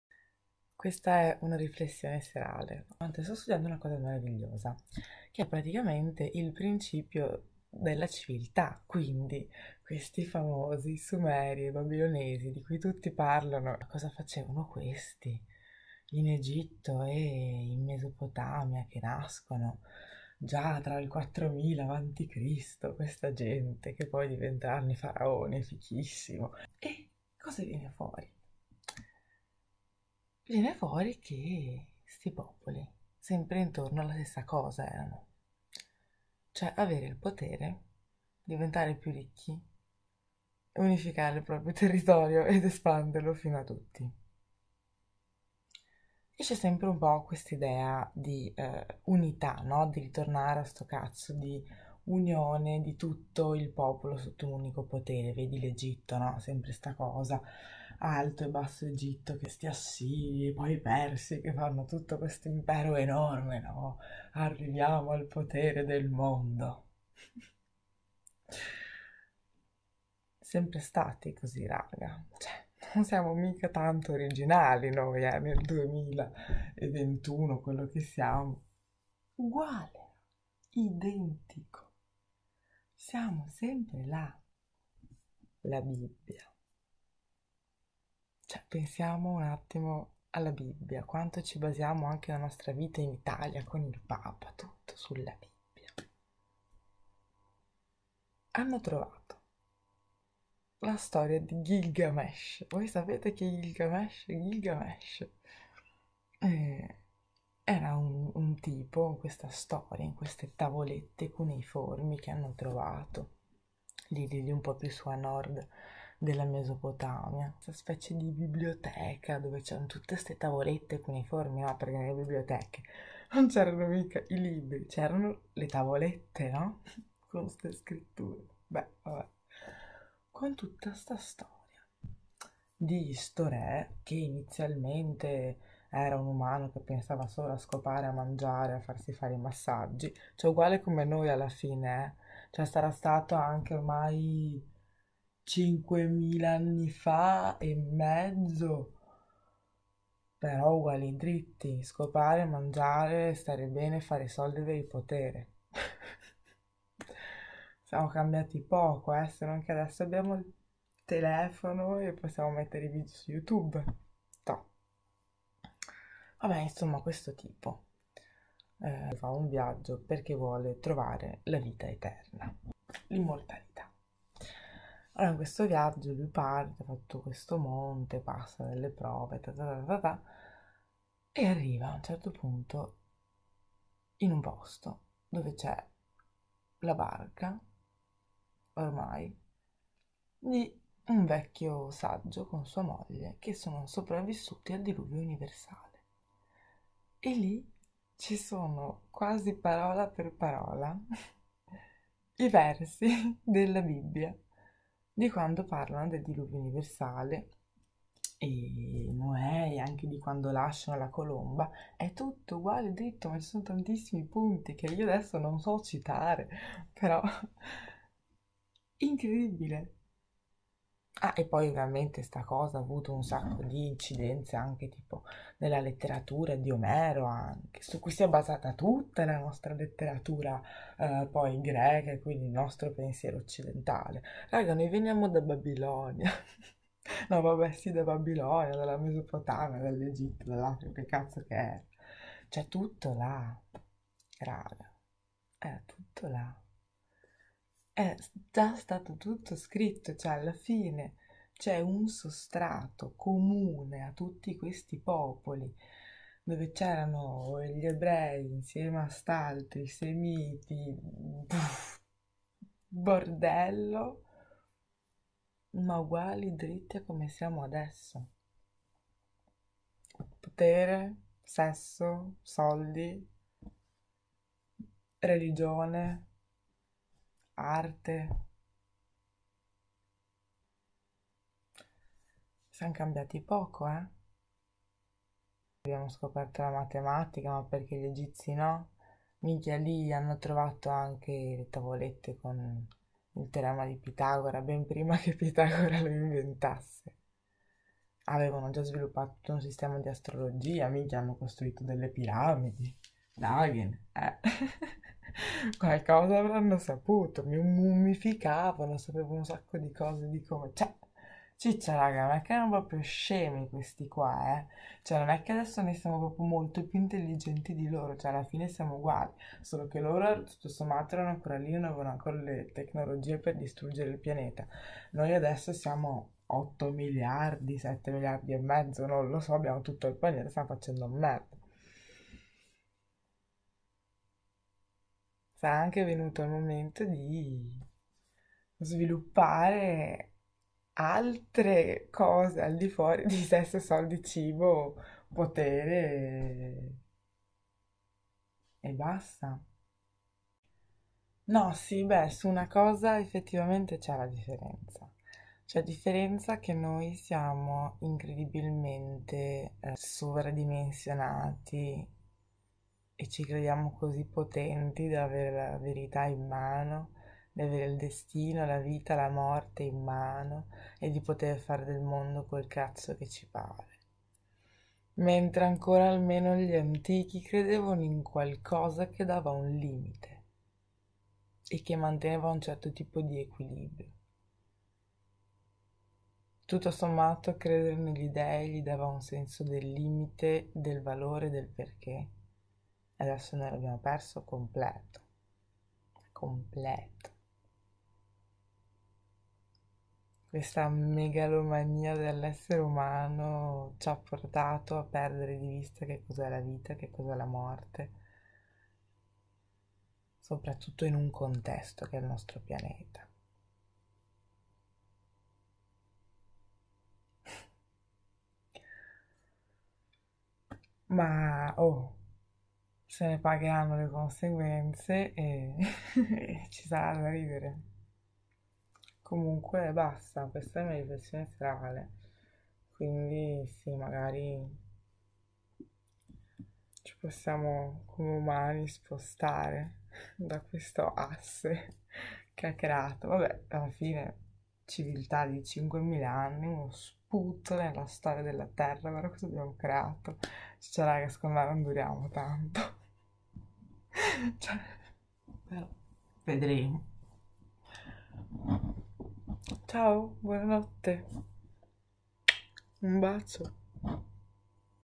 Umani: dall'inizio della civiltà ai giorni nostri...siamo veramente evoluti?
Una piccola riflessione serale sulle differenze che intercorrono tra noi e i primi "uomini civilizzati"... forse non siamo tanto diversi!
Ah, mi sono dimenticata di dire che alla fine, il nostro amico Gilgamesh non troverà la vita eterna, ma troverà la SAGGEZZA. Comprenderà che la conoscenza di sé stesso e dell'uomo è il vero valore della vita.
E tu, cosa ne pensi? Ci siamo veramente evoluti?